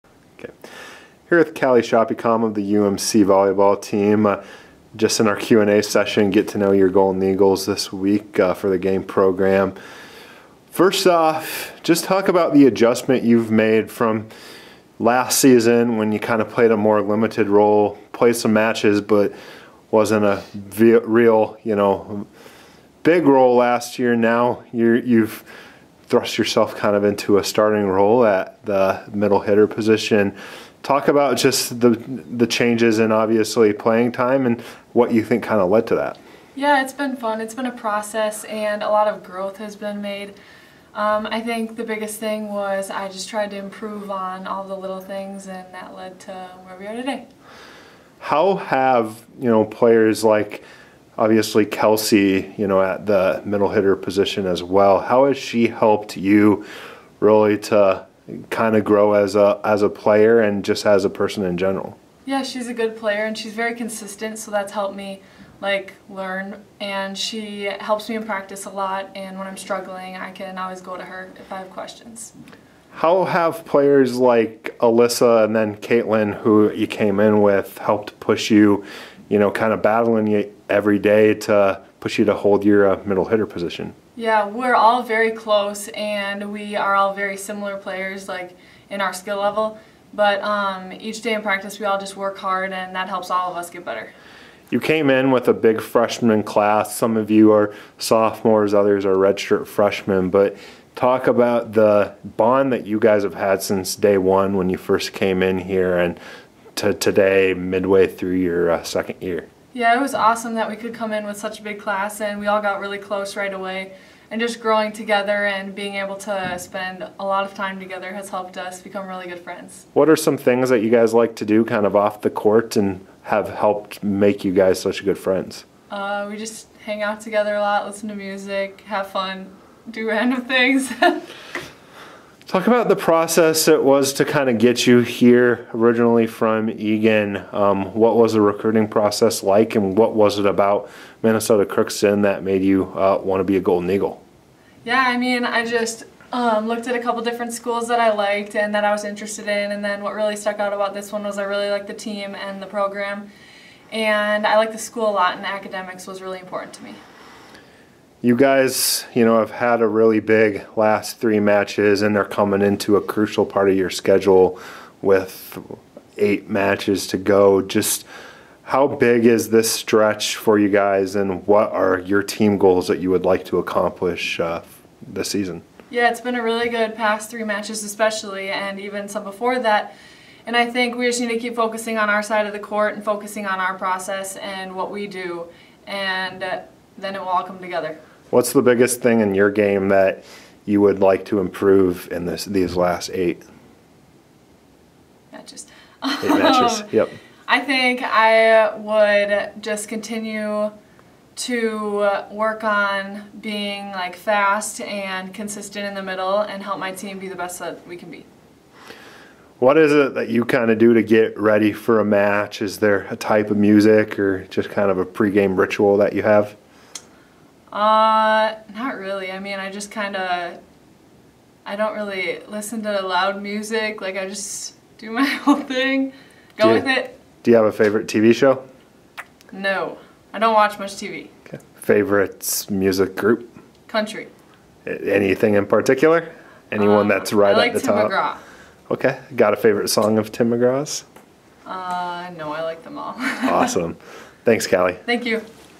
Get To Know Your Golden Eagles: Callie Schapekahm
This week we sat down with UMC volleyball player Callie Schapekahm to get to know her a little better. Callie is a sophomore middle hitter for UMC